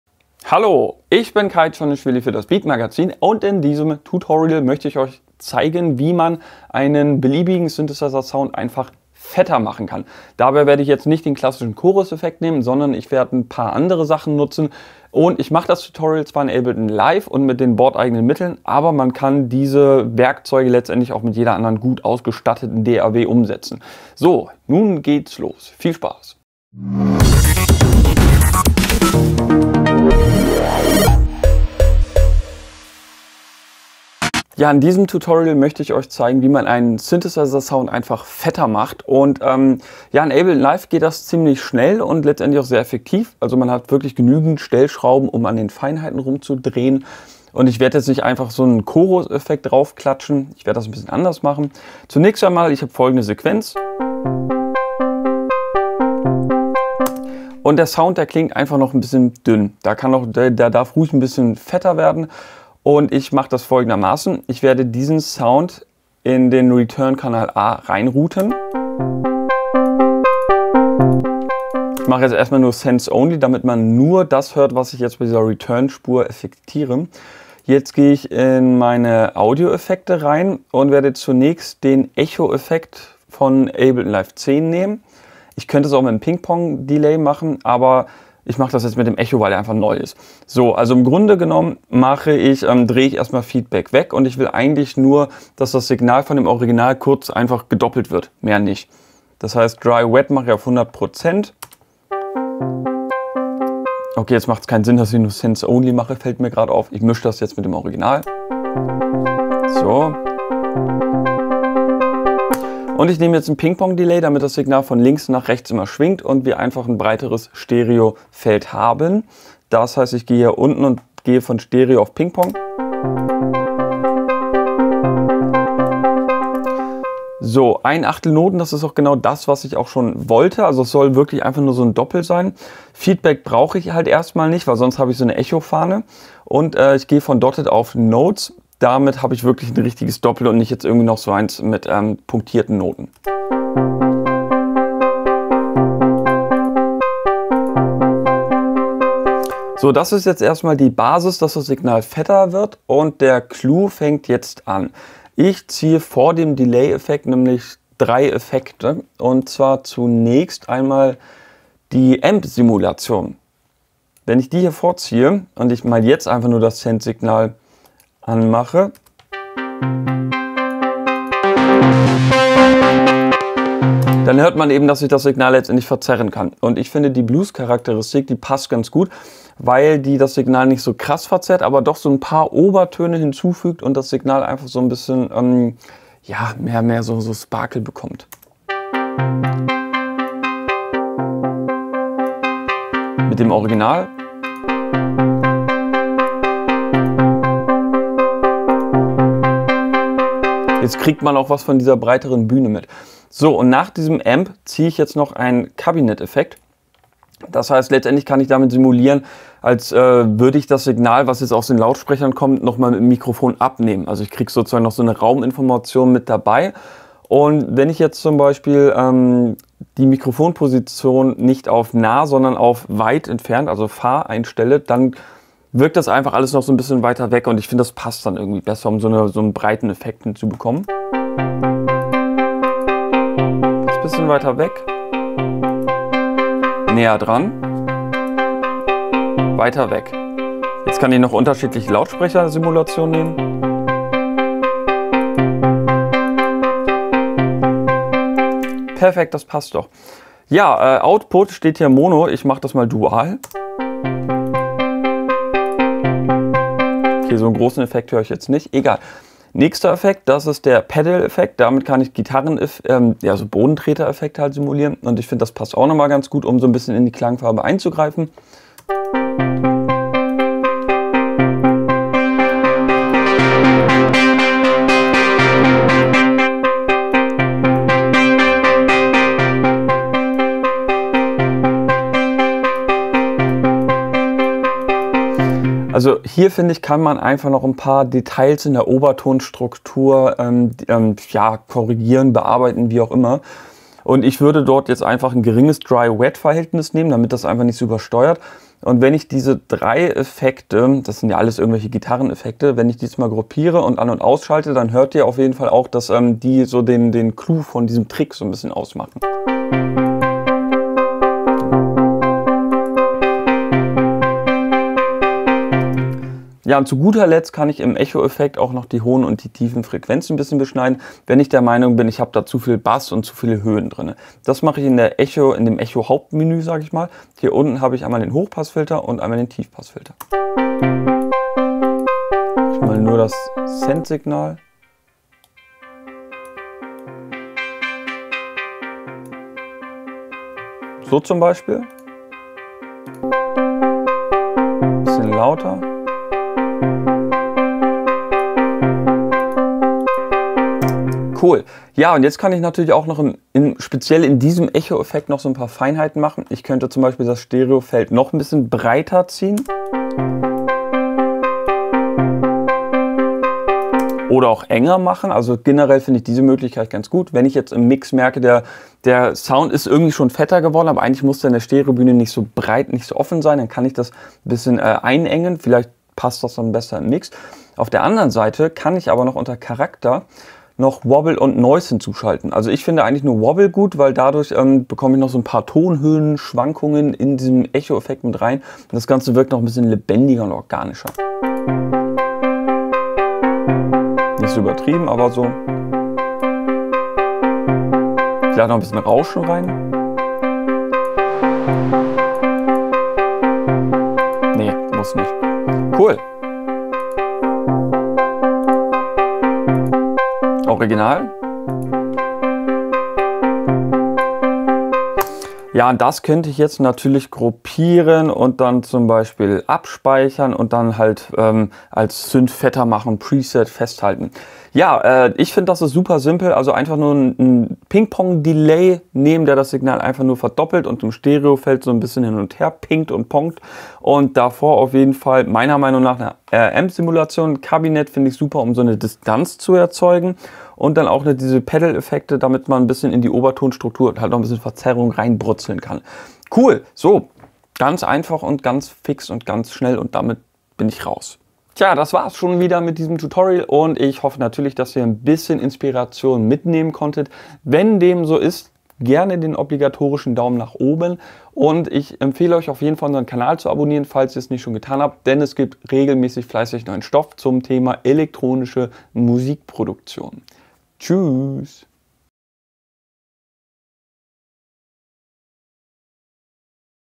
Tutorial: Synthesizer Sound in Ableton Live 10 fetter machen | Producer Tipps #31 | Beat Magazin
Der Sound klingt zu dünn? Im Mix setzt sich der Synthesizer nicht durch? In diesem Tutorial zeigt Kai Chonishvili in Ableton Live 10, wie man einen Synthesizer Sound "fetter" machen kann.

Bei diesem Tutorial kommen Effekte (Delay, Amp Simulation, Cabinet, Distortion) und Send/Return-Wege zum Einsatz, die eigentlich jede DAW haben sollte. Also kann man diesen Sounddesign-Ansatz auch in Bitwig Studio, Studio One, Steinberg Cubase, Apple Logic und so weiter umsetzen. Auch ist dieser Workflow universell, hilft also beim Produzieren von Techno, House, Hip Hop und Co.

Im Detail: ein normaler Synthesizer-Loop wird auf einen externen Delay-Effekt (Send Spur) geroutet. Der Delay-Effekt geht in den Ping-Pong-Modus, um eine breite Stereobühne zu bekommen. Vor dem Delay-Effekt kommen die Ableton-Effekte Amp, Cabinet und Pedal zum Einsatz, sodass die Delays etwas entfernter (Cabinet) und verzerrter sowie durchsetzungsfähiger (Amp, Pedal) wirken. Gemischt mit dem Original-Signal klingt der Synthesizer dann so, als gäbe es eine zweite (Klang)Haut.